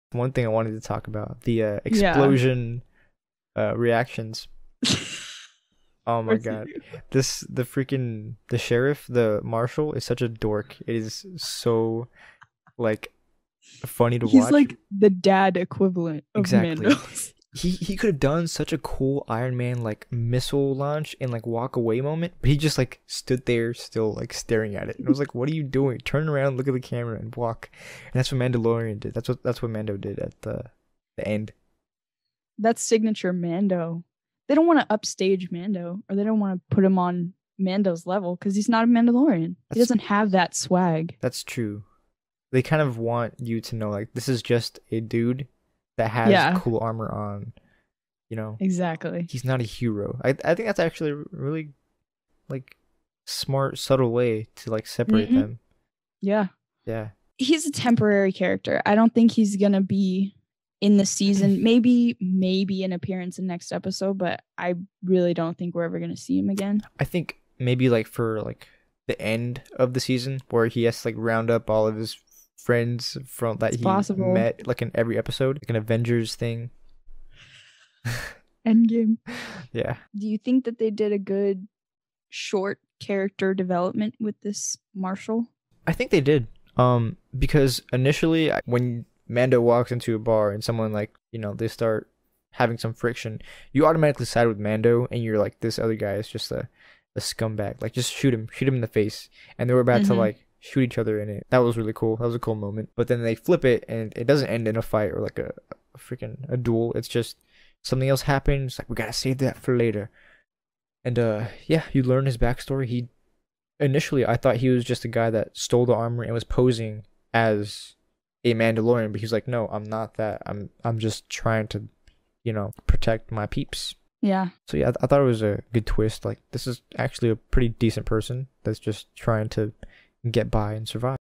The Marshall is a Dork in The Mandalorian
This is a piece from our discussion/podcast type channel and in my opinion the Marshall is a complete dork and looser for his failure at not making cooler poses. Here's the full video!

PATHFINDER FUNKO
Elgato HD Capture Card
Audio Technica AT2020 Mic
Logitch Brio Webcam
REDRAGON Keyboard/Mouse
GT Racing Game Chair